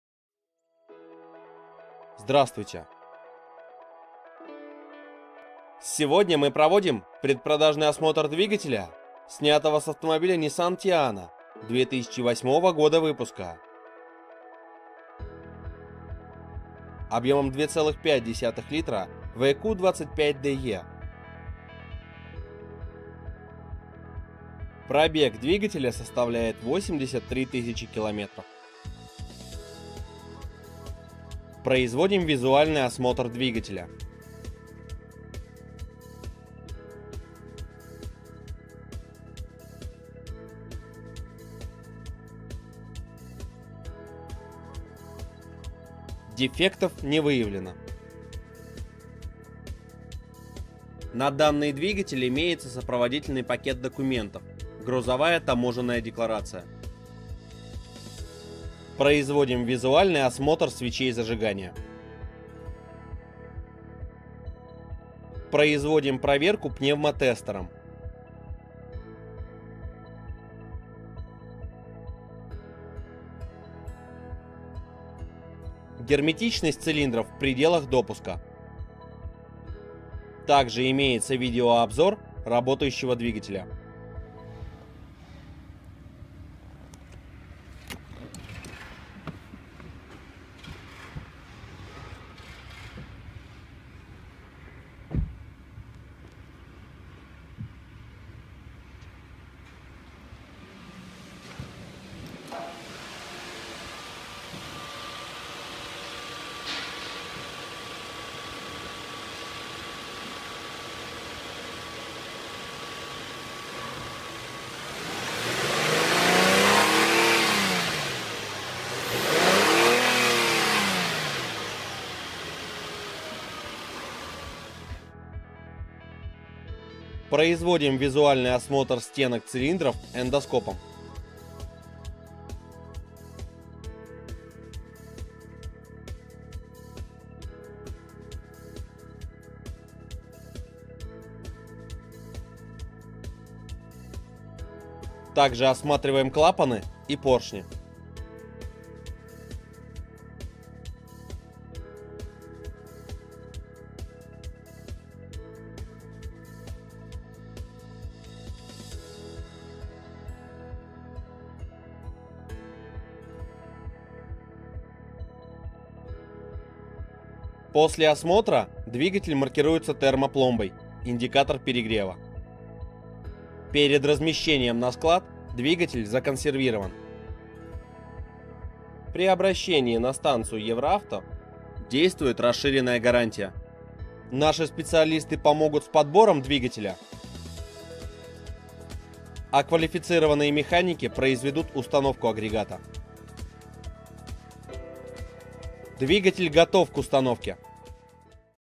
Двигатель Nissan для Teana J32 2008-2013
Описание двигателя Двигатель Nissan для Teana J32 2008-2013
2.5Л. 24V 83000КМ. 2008Г. Q25DE "КОНТРАКТНЫЙ" ИЗ ЯПОНИИ (СОПРОВОДИТЕЛЬНЫЙ ПАКЕТ ДОКУМЕНТОВ: СМР, ГТД, ДКП) ИМЕЕТСЯ ВИДЕО РАБОТЫ ДВИГАТЕЛЯ 10102JN0A1